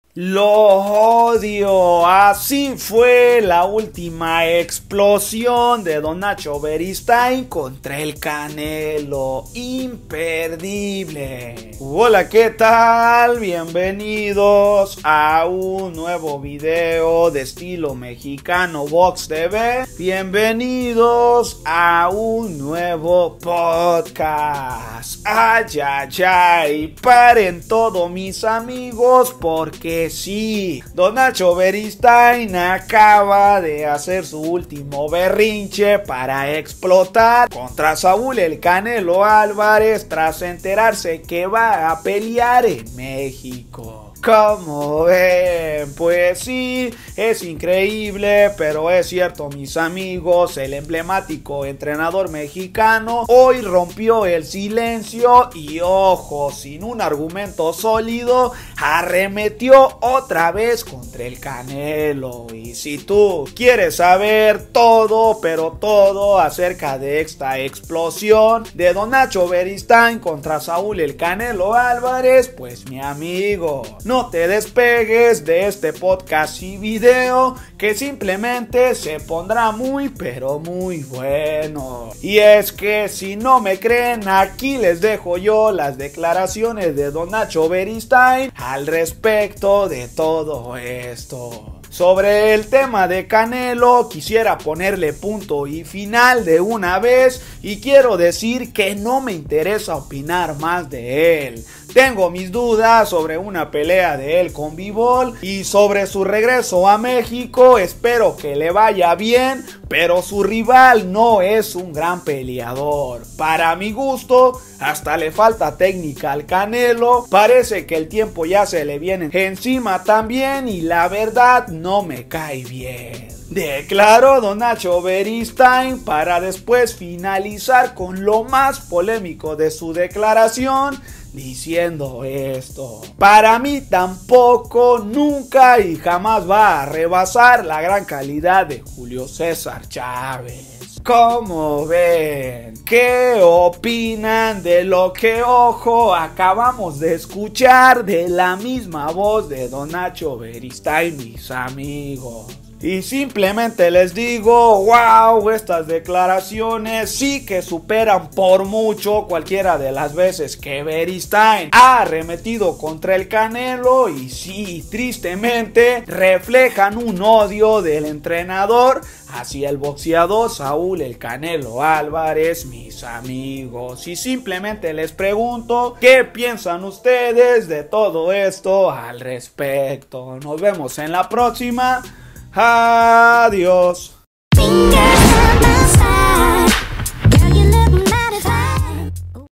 ¡LO ODIO! Asi fue la ultima EXPLOSION de Don Nacho Beristain contra Canelo (IMPERDIBLE) / Podcast
Don Nacho EXPLOTÓ contra Canelo‼️